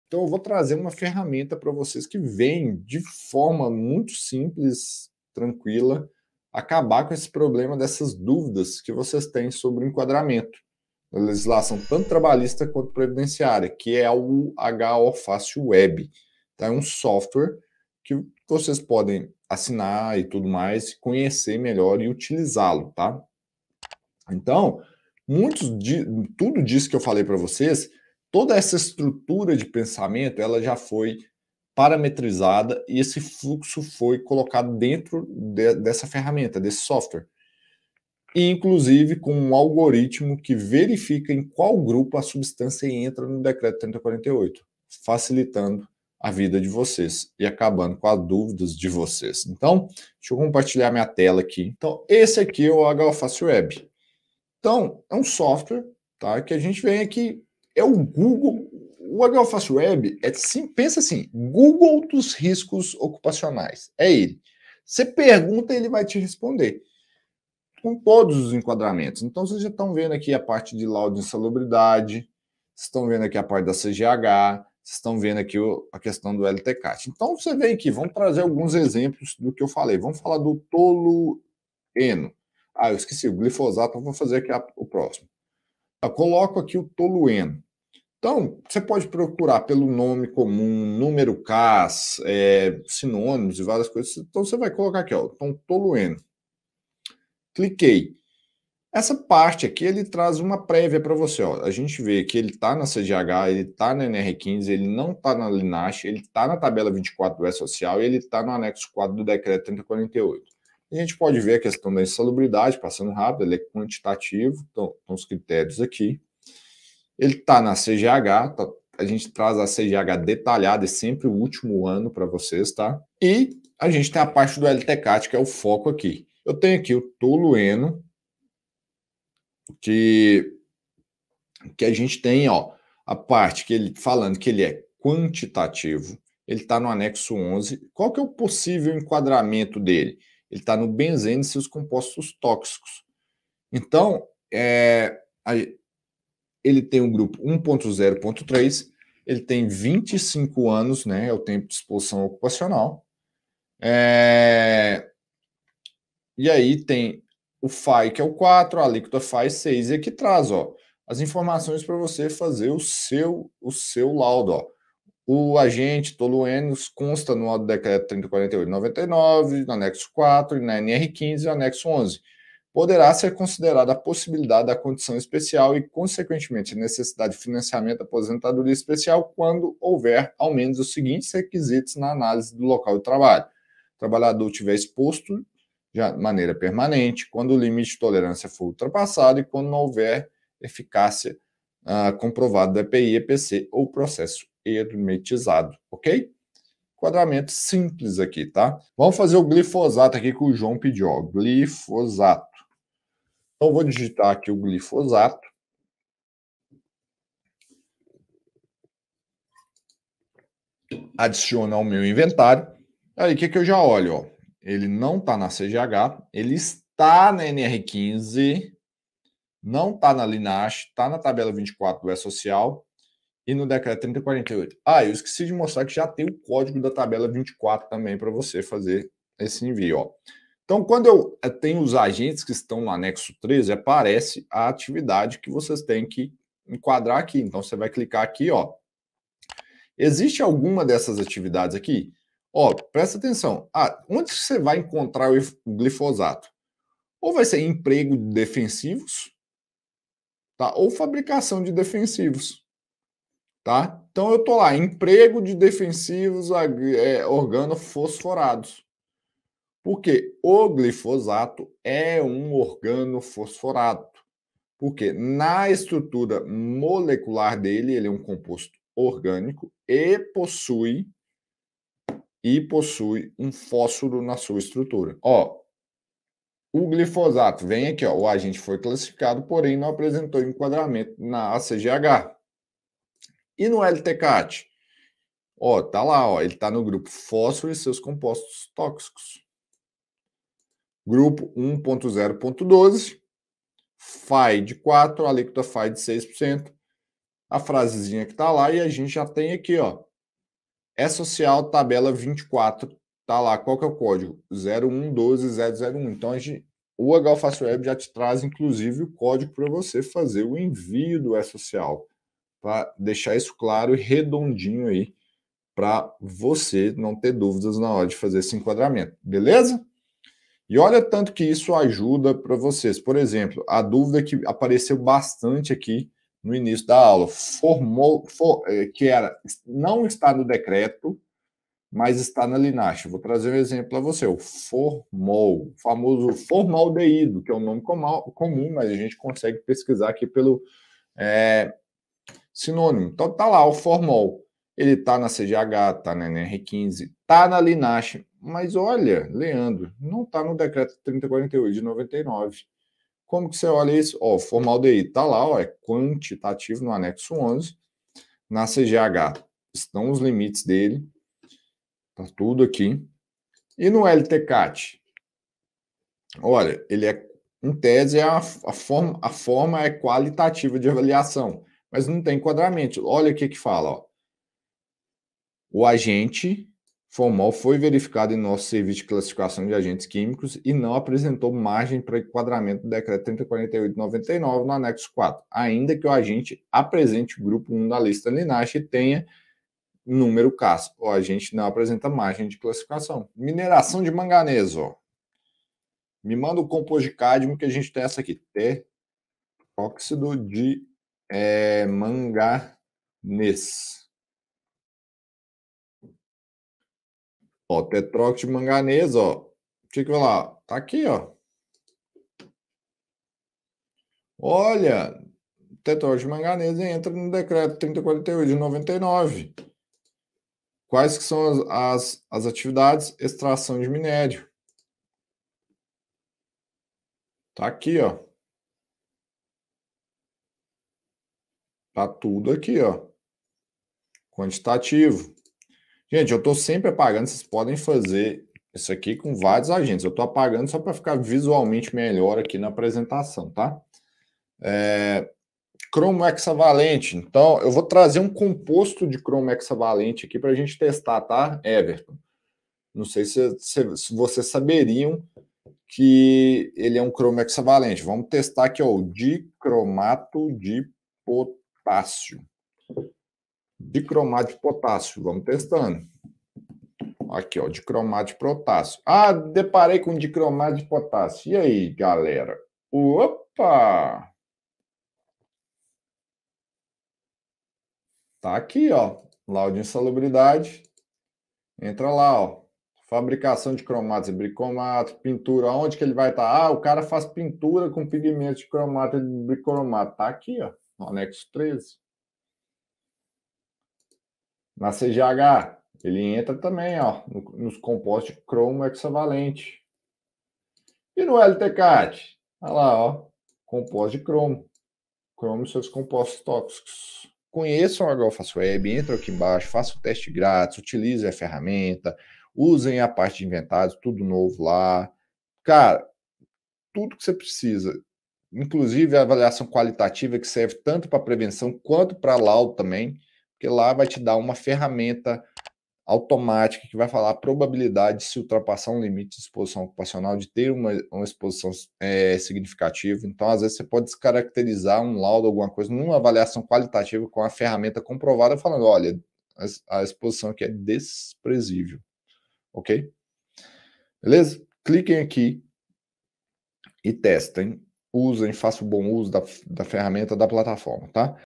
O modo de pesquisa rápida para saber se o agente se enquadra na aposentadoria especial - HO #69
Identificar um se um agente químico enquadra para aposentadoria especial não precisa ser um pesadelo. Neste vídeo, eu vou te mostrar uma ferramenta que irá facilitar o enquadramento dos agentes químicos na legislação previdenciária para fazer os envios para o eSocial, acompanha tudo até o final.